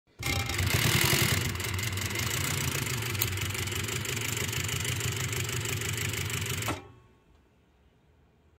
Consew 226r-2 won't sew
another video of my machine not Sewing.  number 92 thread  135/17 size 18 needle.  will not sew.  I've been trying to fix this for two weeks.  I'm going to smash this thing to pieces.  or melt it down to a  block of iron.